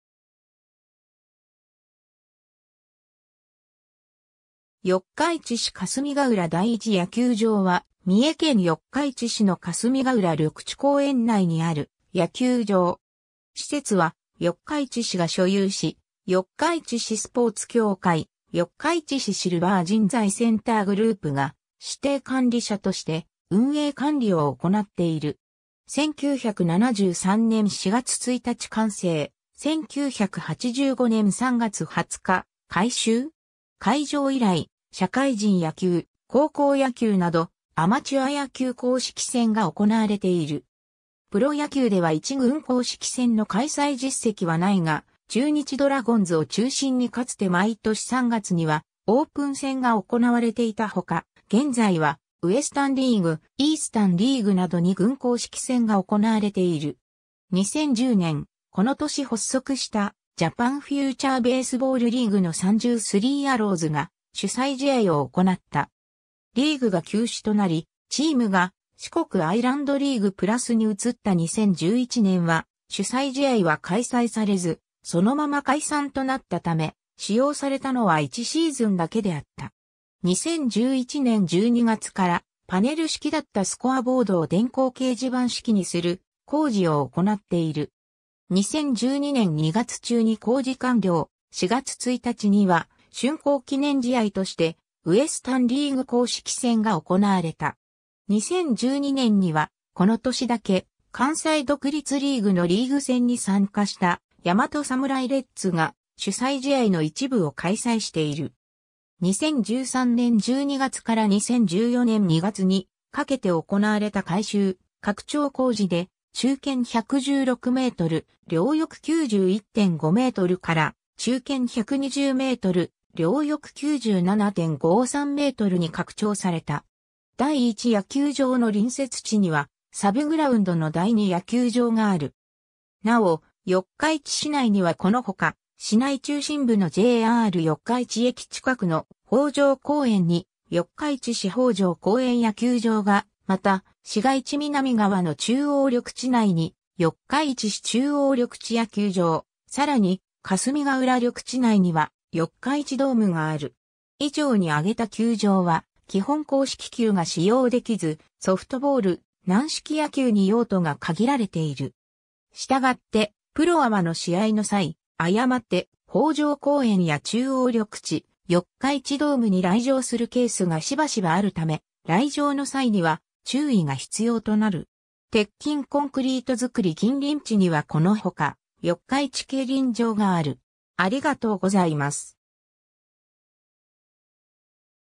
四日市市営霞ヶ浦第一野球場
四日市市霞ヶ浦第一野球場（よっかいちし・かすみがうら・だいいちやきゅうじょう）は、三重県四日市市の霞ヶ浦緑地公園内にある野球場。
施設は四日市市が所有し、四日市市スポーツ協会・四日市市シルバー人材センターグループが指定管理者として運営管理を行っている。
 1973年（昭和48年）4月1日完成、1985年（昭和60年）3月20日改修。
開場以来、社会人野球、高校野球などアマチュア野球公式戦が行われている。
プロ野球では一軍公式戦の開催実績はないが、中日ドラゴンズを中心にかつて毎年3月にはオープン戦が行われていた他、現在はウエスタン・リーグ、イースタン・リーグなど2軍公式戦が行われている。
 2010年、この年発足したジャパン・フューチャーベースボールリーグの三重スリーアローズが主催試合をおこなった。
リーグが休止となり、チームが四国アイランドリーグplusに移った2011年は主催試合は開催されず、そのまま解散となったため、使用されたのは1シーズンだけであった。
 2011年12月からパネル式だったスコアボードを電光掲示板式にする工事を行っている。
2012年2月中に工事完了、4月1日には竣工記念試合としてウエスタン・リーグ公式戦（中日VSオリックス）が行われた。
 2012年には、この年だけ関西独立リーグのリーグ戦に参加した大和侍レッズが主催試合の一部を開催している。
 2013年12月から2014年2月にかけて行われた改修・拡張工事で、「中堅116メートル・両翼91.5メートル」から「中堅120メートル・両翼97.53メートル」に拡張された。
 第一野球場の隣接地にはサブグラウンドの第二野球場がある。
 なお四日市市内にはこの他、市内中心部のJR四日市駅近くの北条公園に四日市市北条公園野球場が、また市街地南側の中央緑地内に四日市市中央緑地野球場、さらに霞ヶ浦緑地内には四日市ドームがある。
以上に挙げた球場は、基本硬式球が使用できず、ソフトボール・軟式野球に用途が限られている（四日市ドームは天井が低いため、軟式球は少年野球のみに限定しており、成人用の試合は不可）。
従ってプロ・アマの試合の際、誤って北条公園や中央緑地、四日市ドームに来場するケースがしばしばあるため、来場の際には注意が必要となる。
 鉄筋コンクリート造 ※ 近隣地にはこの他、四日市競輪場がある。